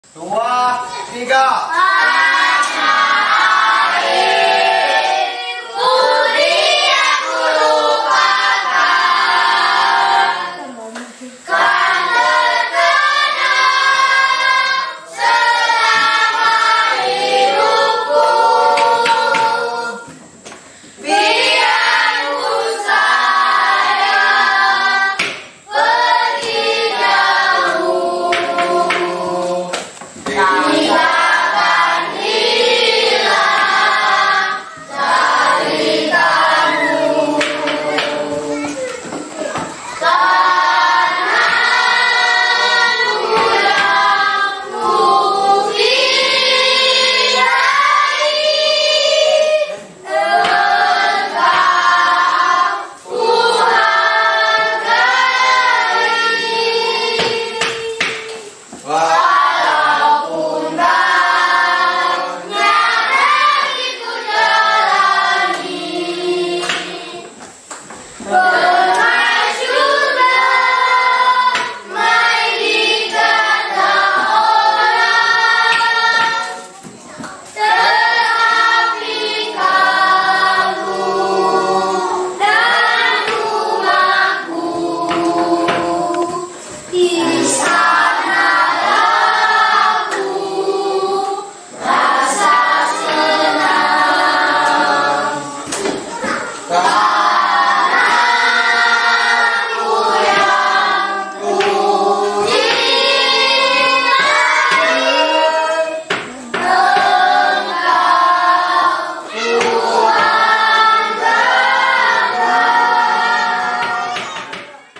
Merinding..!! Siswa SD di Perbatasan Indonesia-Malaysia Menyanyikan Ini
Satgas Pamtas RI-MLY Yonif 407/PK mengajarkan siswa siswi di SD Perbatasan menyanyikan lagi nasional tanah air